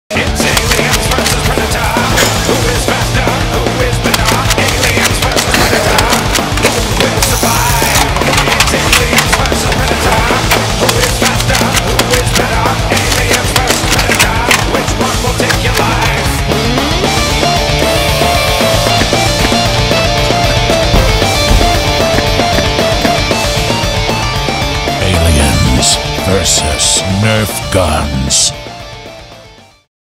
EVENT ANNOUNCEMENT: Aliens Vs Nerf Guns - May 18 & 19
Shoot aliens in the Bay Area at this interactive, live-action event! You and your friends must survive an alien-infested haunted house with nerf guns!